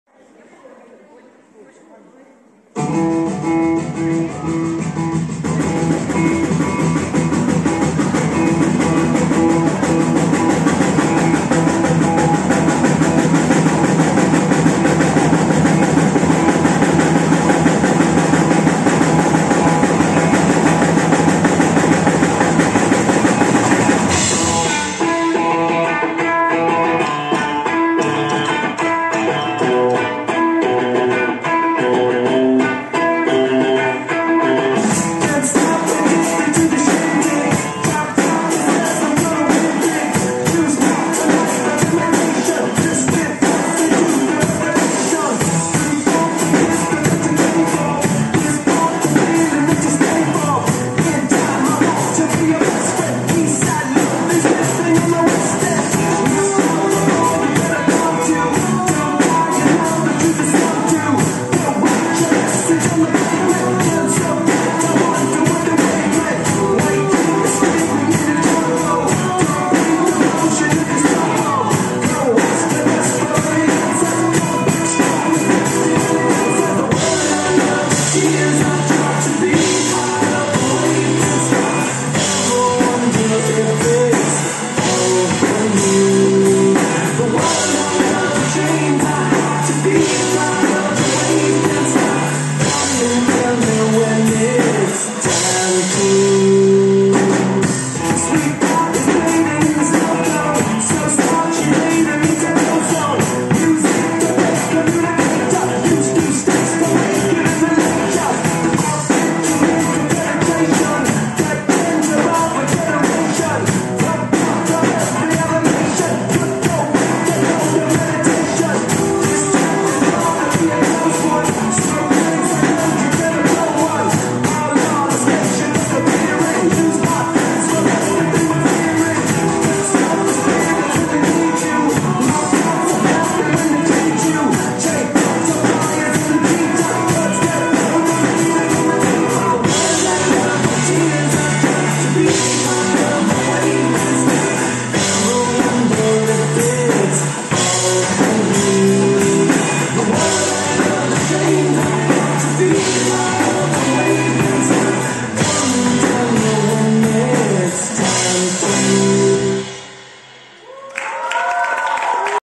Затірка Іван - Red Hot Chilli Pappers
Благодійний концерт Drum_Art_Music_School "Мистецтво заради життя" (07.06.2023)